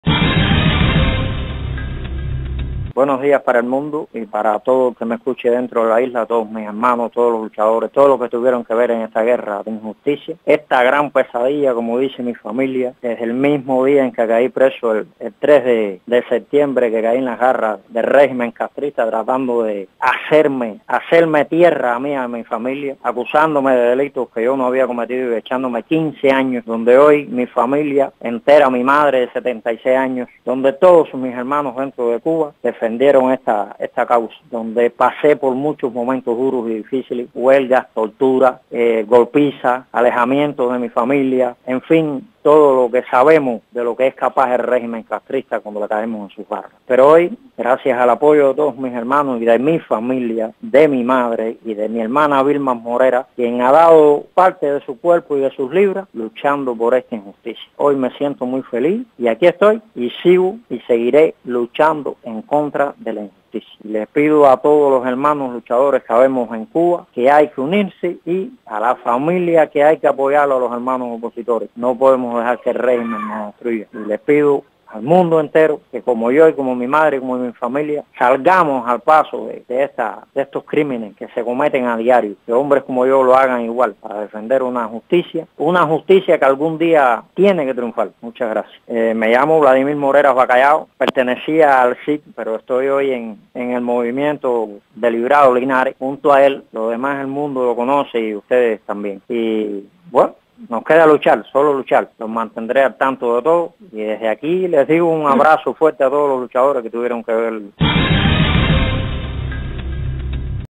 Declaración de Vladimir Morera Bacallao
Matanzas, 14 de octubre del 2014- Activista de DDHH  del Movimiento Cubano de Reflexión, Vladimir Morera Bacallao declara sobre su detención, golpiza y huelga de hambre y agradece el triunfo de su huelga de hambre a su hermana, madre, familia y activistas en la oposición al regimen castrista dentro y fuera de Cuba que se solidarizaron con su caso.